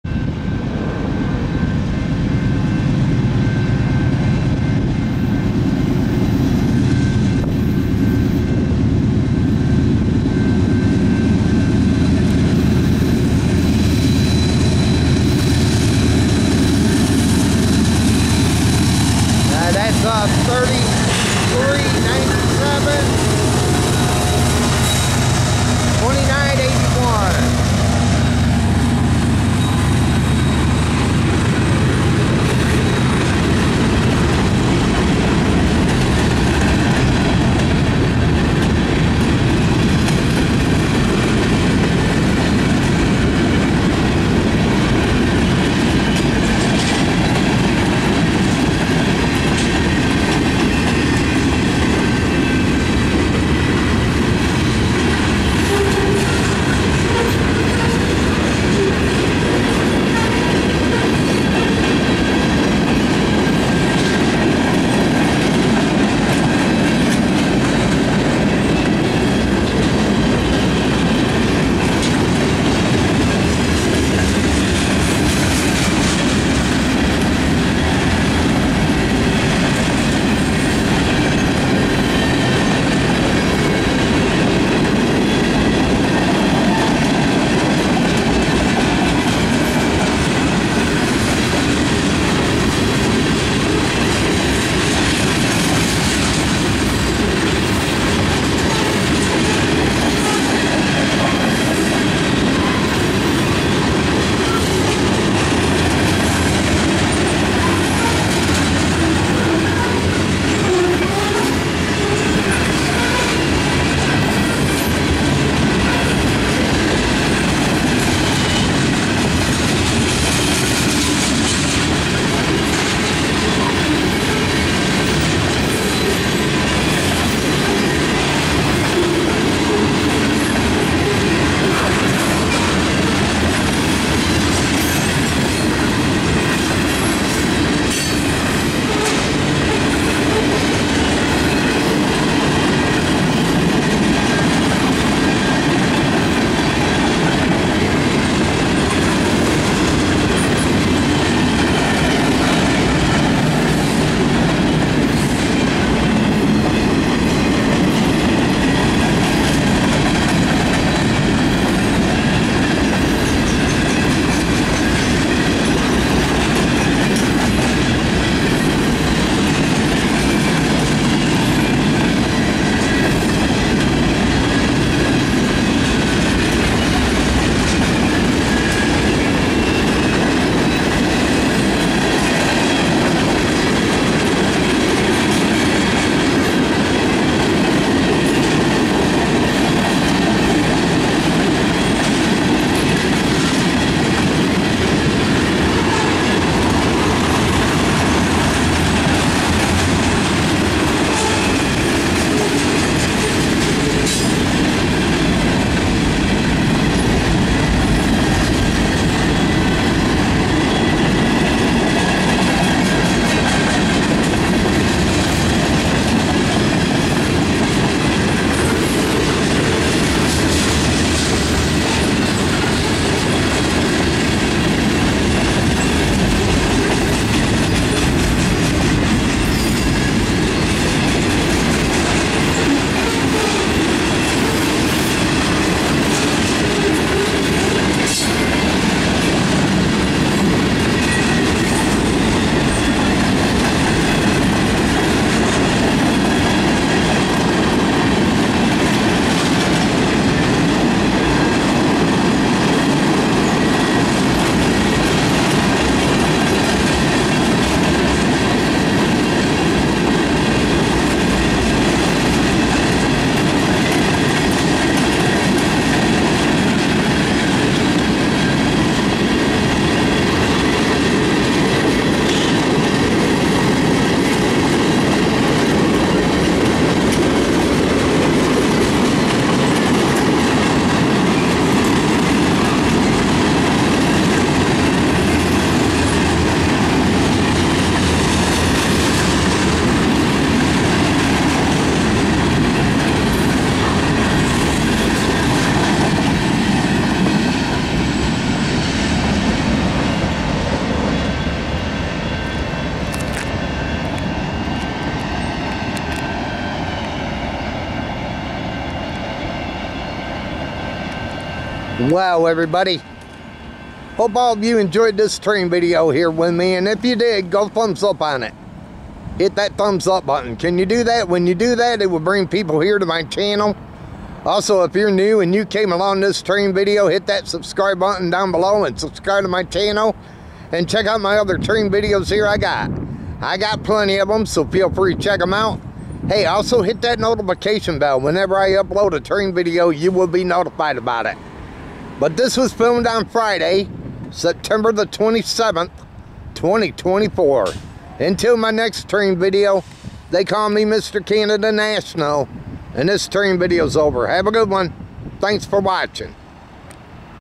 CN Train A491 Heading Westbound Battle Creek Michigan 9/27/24
Railfanning From The Amtrak Station

CN 3397
CN 2981